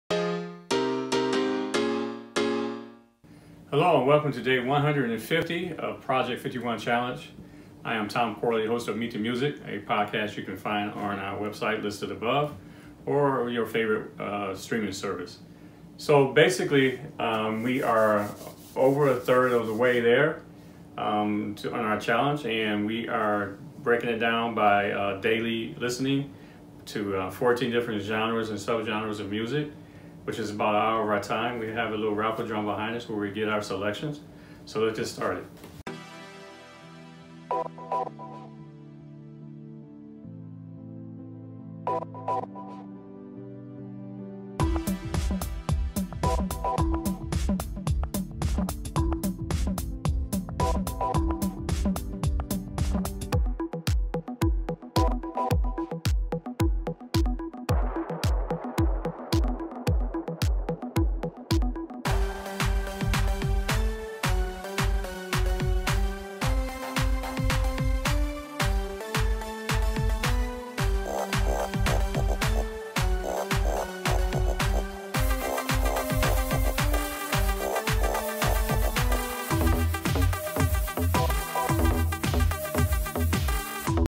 Project 5112 Challenge - 365 Days of New Music Genres - Day 150
Join the Project 5112 Challenge as we aim to listen to 5,112 music genres in 365 days.  Join our musical journey.  Watch the daily drawing of 14 different music genres.